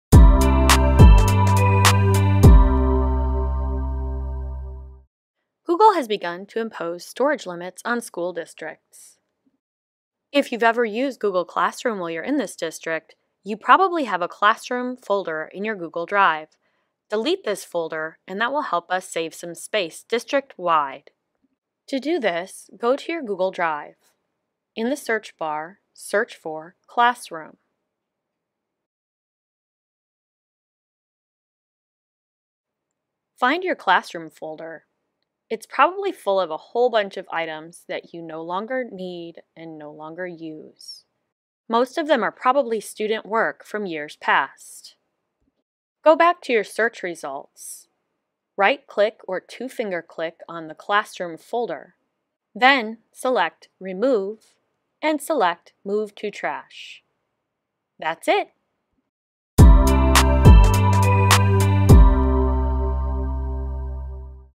"Classroom" Folder in Your Google Drive
Google is imposing storage limits on school districts. Help us stay under the limit by removing your old Google Classroom folder titled "Classroom."